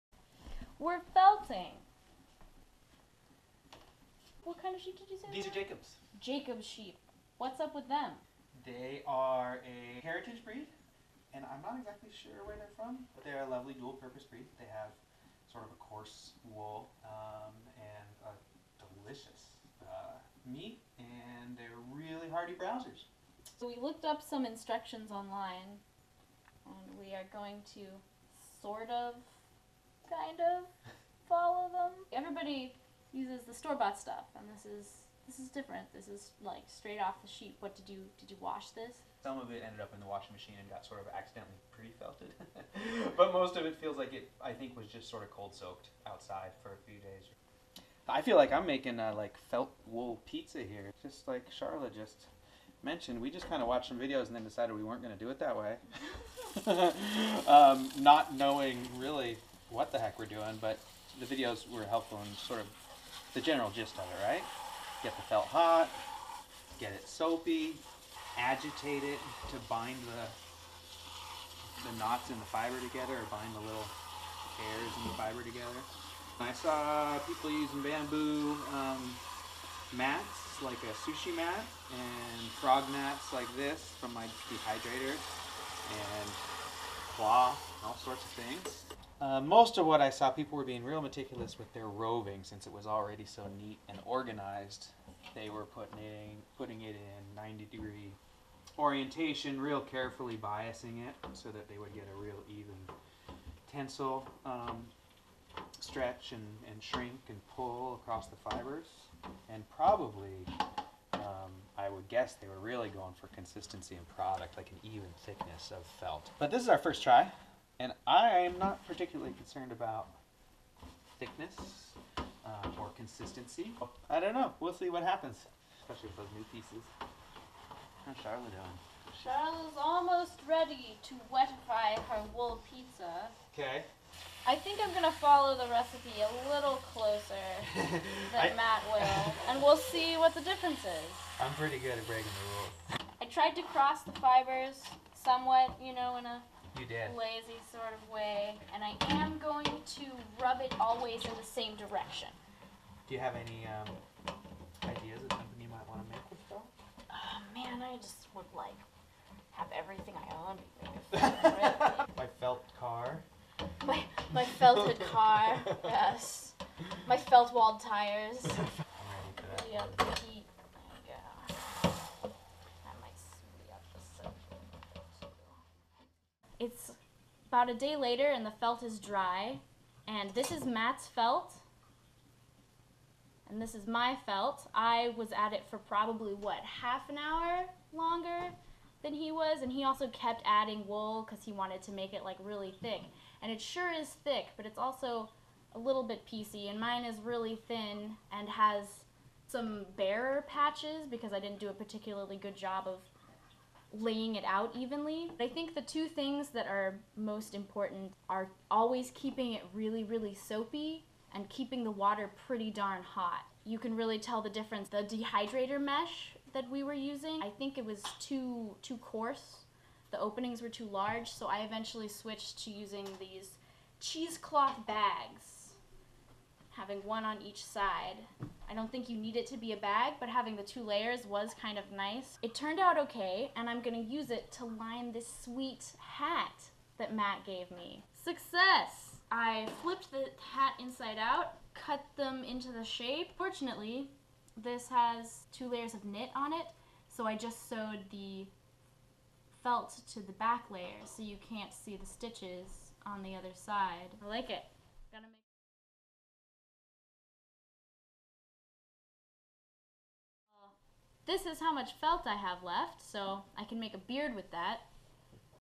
Felting with Matt
We spent a glorious week and a bit at Matt Walker's awesome homestead on the Olympic Peninsula and made some felt!